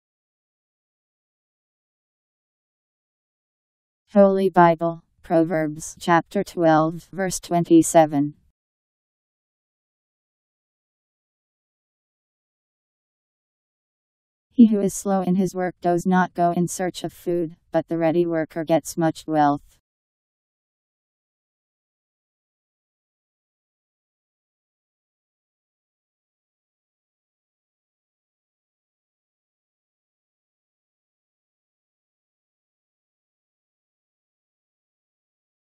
Holy Bible Proverbs 12:27
Video shows Holy Bible Proverbs Chapter 12 Verse 27: He who is slow in his work does not go in search of food; but the ready worker gets much wealth. Powered by MaryTTS. Text taken from Bible in Basic Edition. Bible Proverbs 12:27